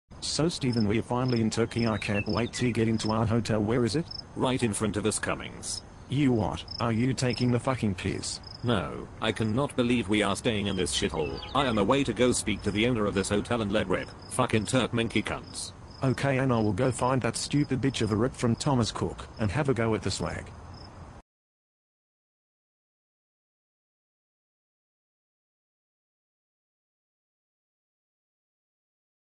Cummings And Steven Go To Turkey
This is part one of cummings and stevens rubbish trip abroad